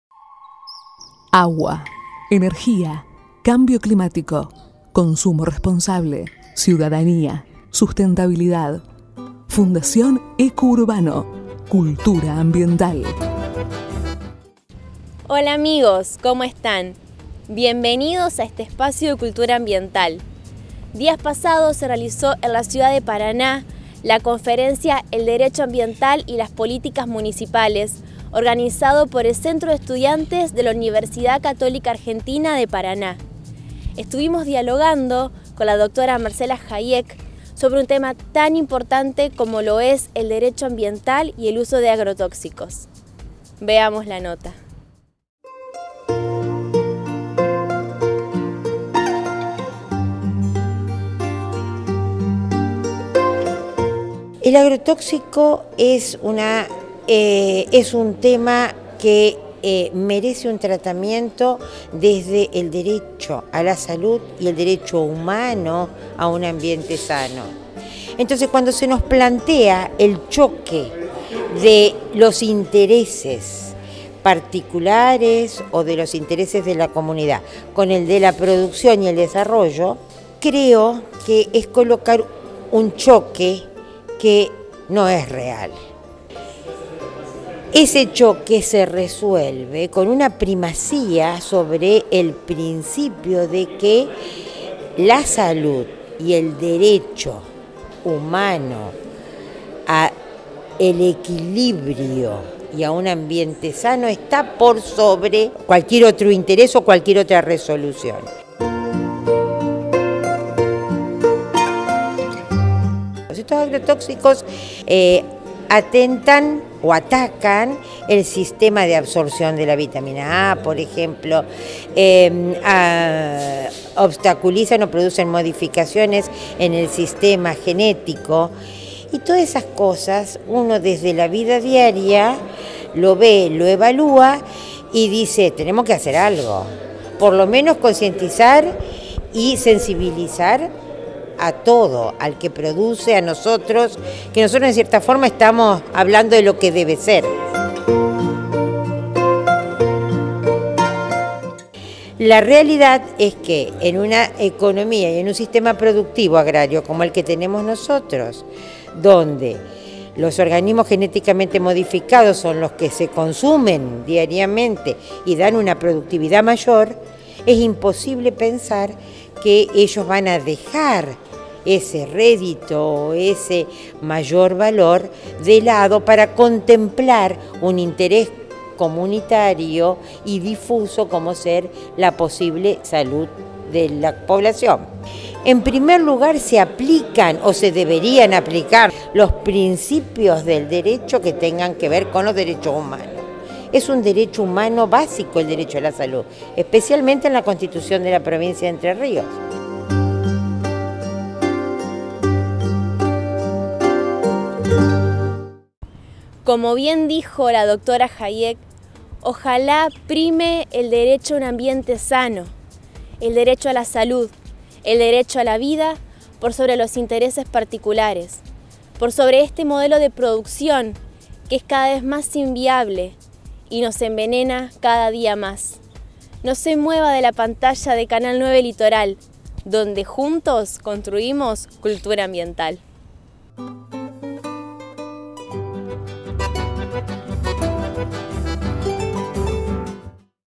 El Derecho humano a la salud y el uso de agrotoxicos
Días pasados se realizó en la ciudad de Paraná la Conferencia “El Derecho Ambiental y las Políticas Municipales". La misma fue organizada por el Centro de Estudiantes de Derecho de la Universidad Católica Argentina Paraná.

Dialogamos con una de los conferencistas, la Dra. Marcela Haiek, sobre un tema tan importante como es el derecho ambiental y el uso de agrotóxicos…